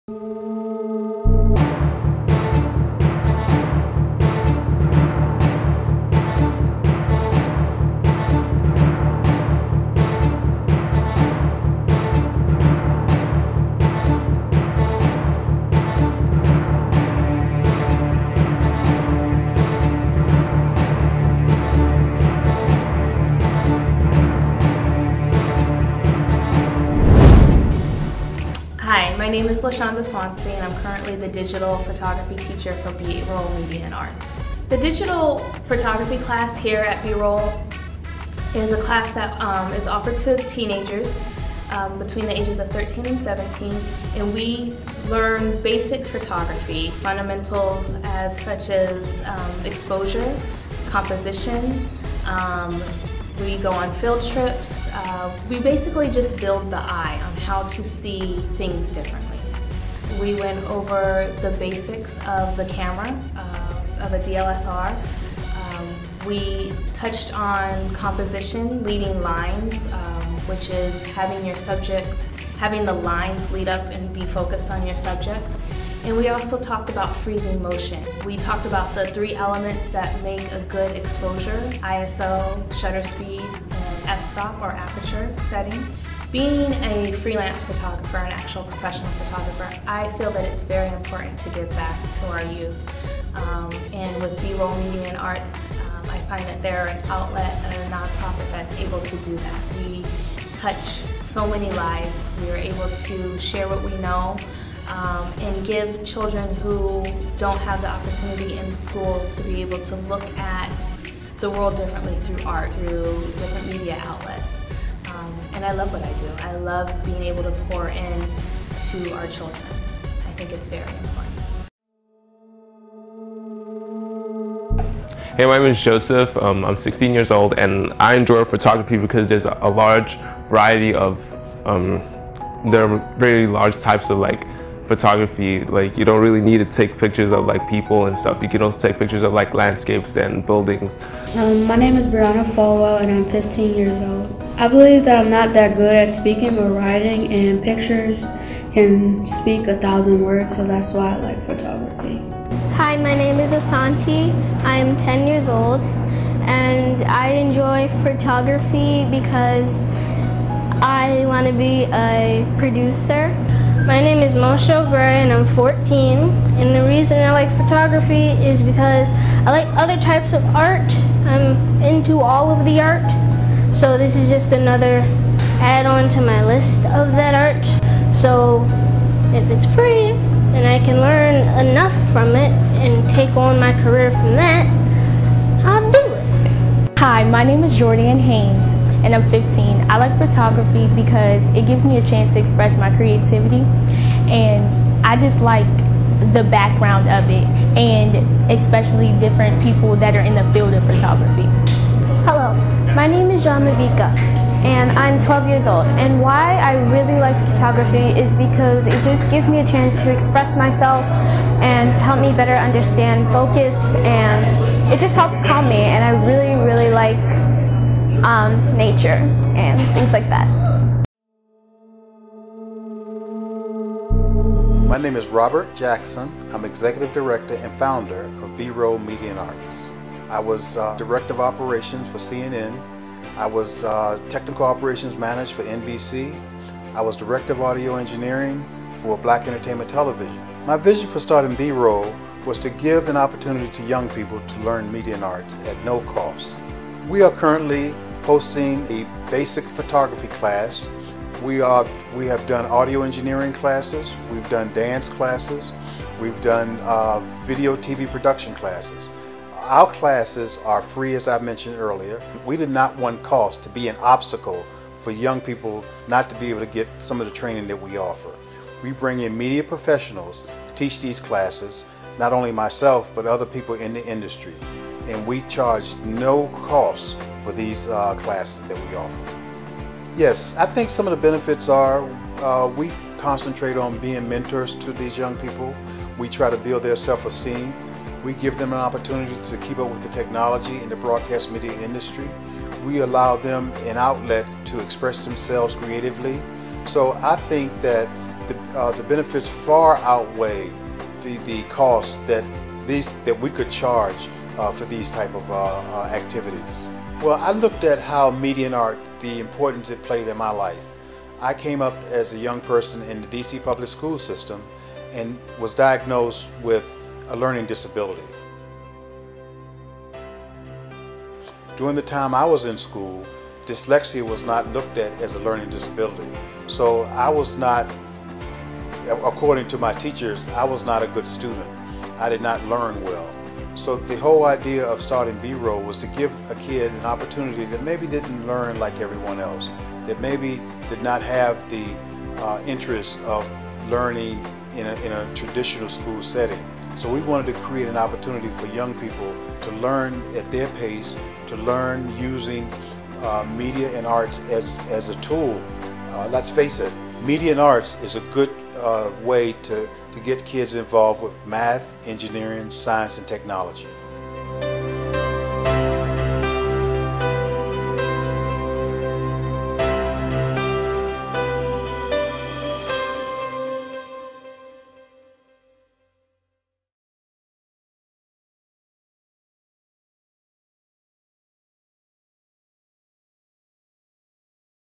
Final interview 2
B-Roll Photography Class summer 2016.

Robert Jackson talks about why he started B-Roll Media & Arts.